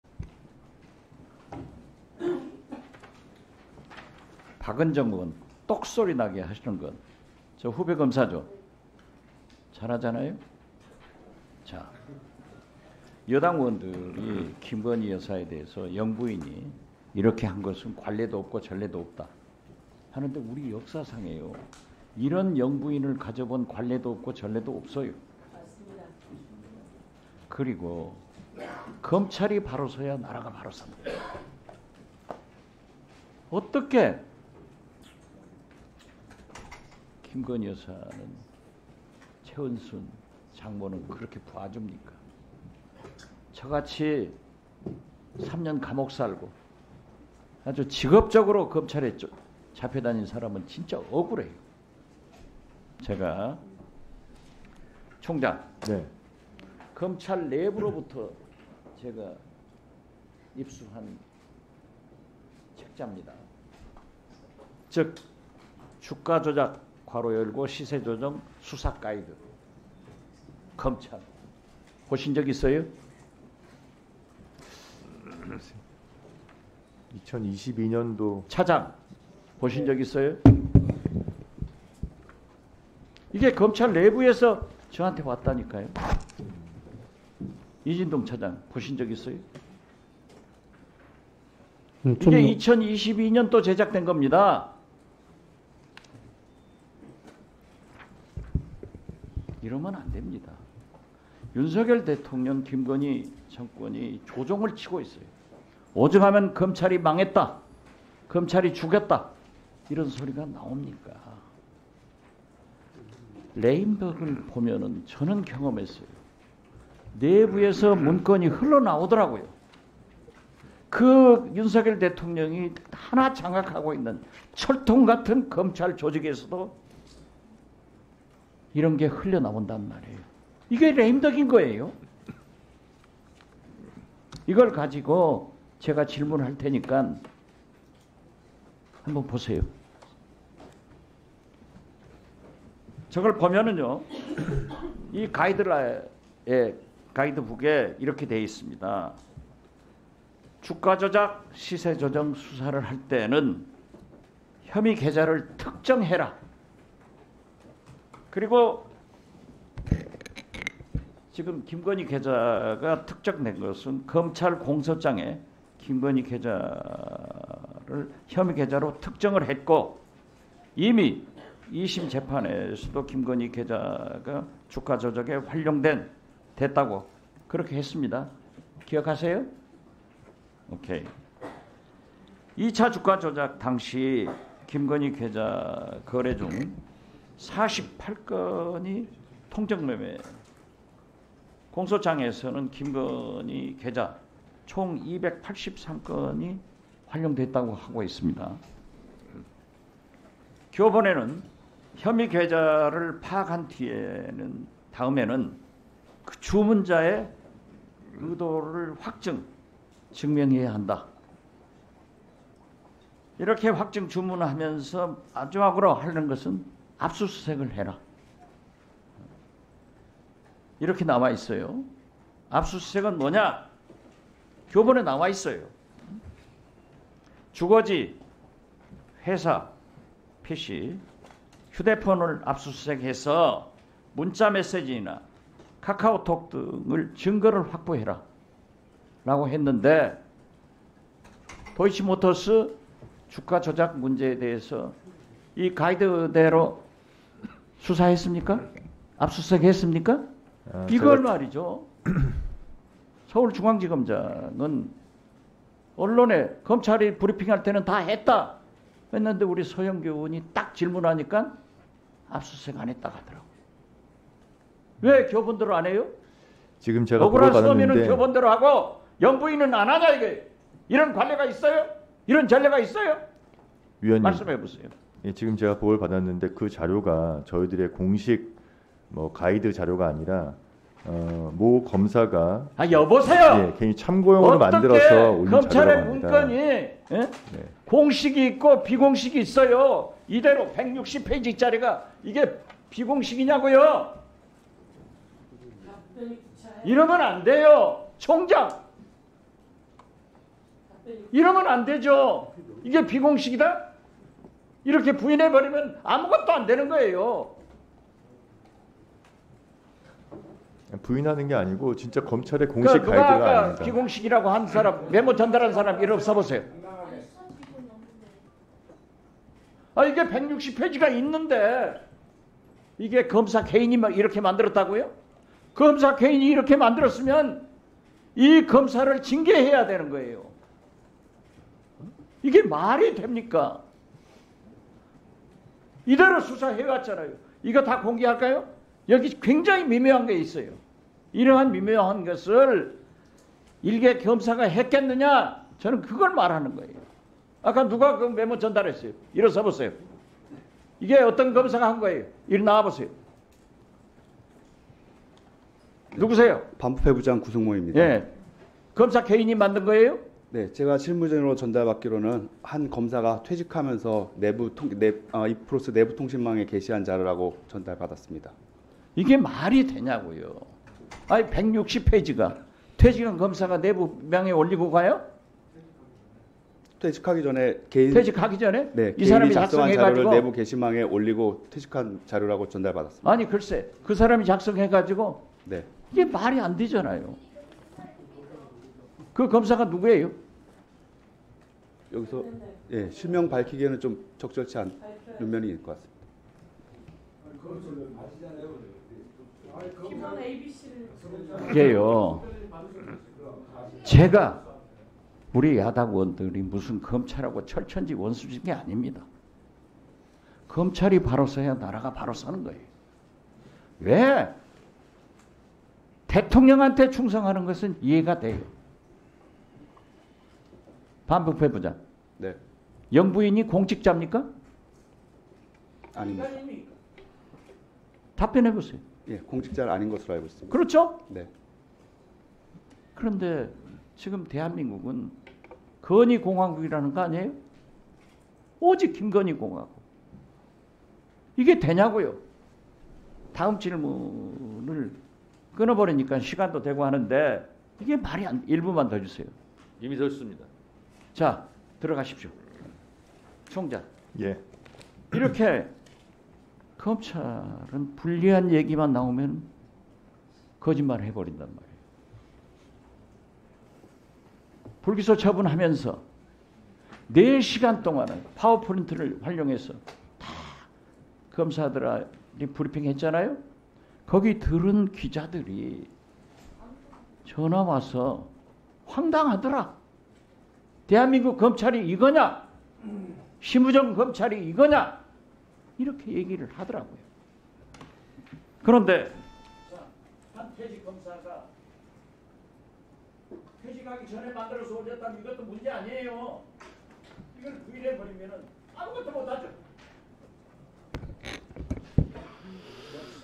검찰 수사 가이드 지적하는 박지원 의원 "퇴직검사가 참고용으로 만들었다?" : 법사위 국정감사 : 241021
국회 법제사법위원회는 오늘 대검찰청을 대상으로 2024년도 국정감사를 실시합니다. 윤석열 대통령 부인 김건희 여사의 `공천개입 의혹`을 제기한 강혜경 씨가 증인으로 출석합니다. 도이치모터스 주가조작